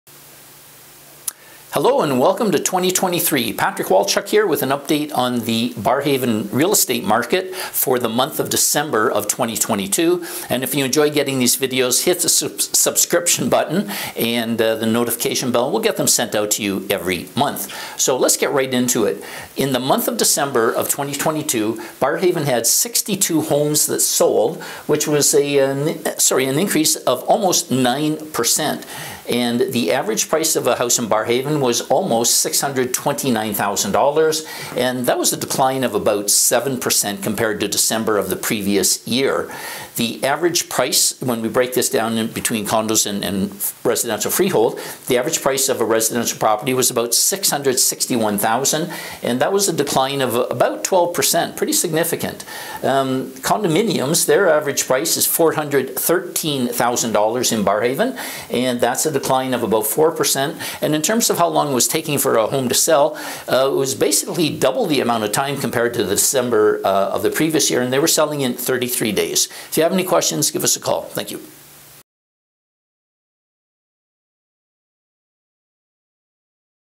Barrhaven Market Update - Dec. 2022
Housing Market Update for Barrhaven, January 7th, 2022: There were a total of 62 home sales in Barrhaven in December 2022, up by 8.8%. 54 sales were freehold homes, up 22.7% and 8 sales were condos, versus 13 last year.

🏠💲 The average price for a Freehold House in Barrhaven in December was $660,771, down by 11.8%. This includes detached houses, townhomes and semi-detached homes.

🏙💲 The average price for a condo in Barrhaven in December was $413,125, down by 4%. This includes all types of condos, apartments, townhomes and stacked style.

Last month it took 33 days for homes to sell in Barrhaven, versus 13 days last year.